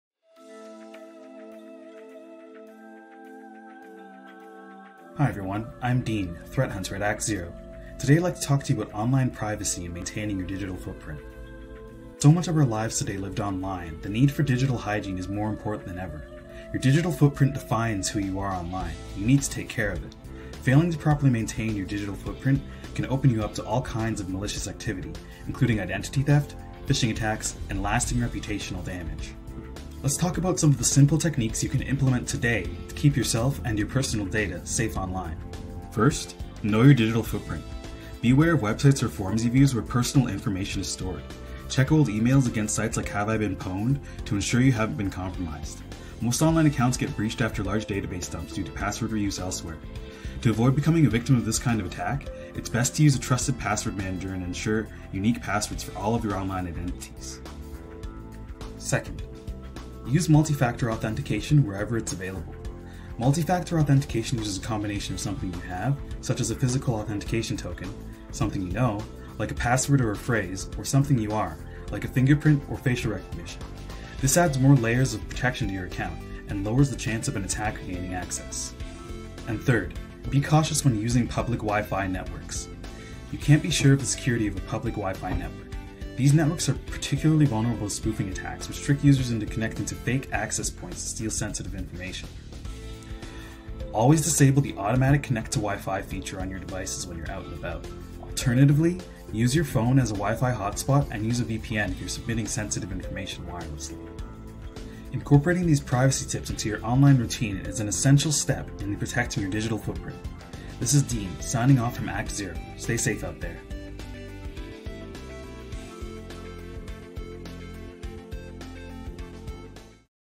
Tips From Our Threat Hunters: Online Privacy & Maintaining Your Digital Footprint
Threat Hunter, Dean Williams, gives three tips to better maintain your online privacy and digital footprint.